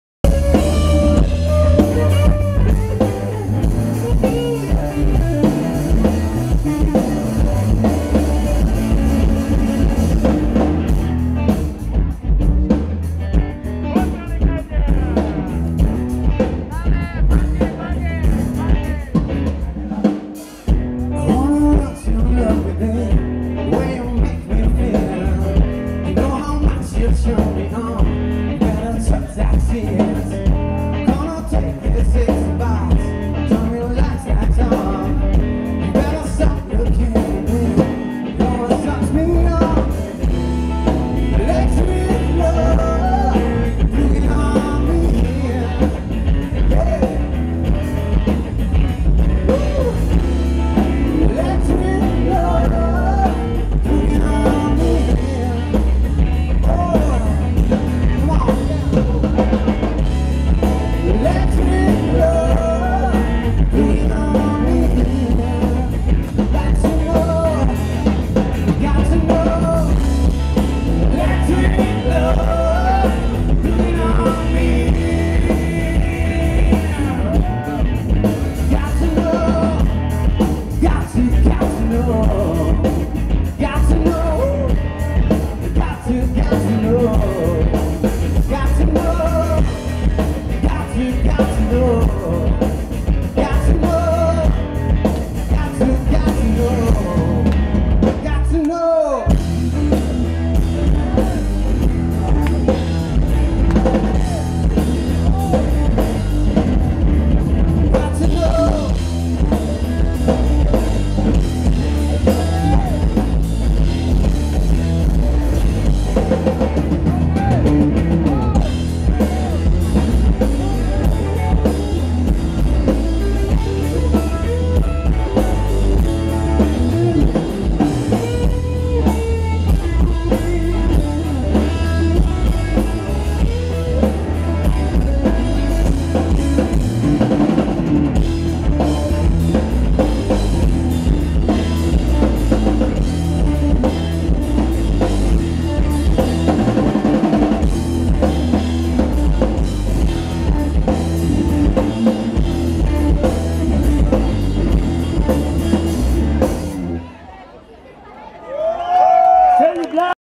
Sergi Blanch TRIO
Actuació de Sergi Band Trío a Cadaques, la tarda del dissabte 11 de juny.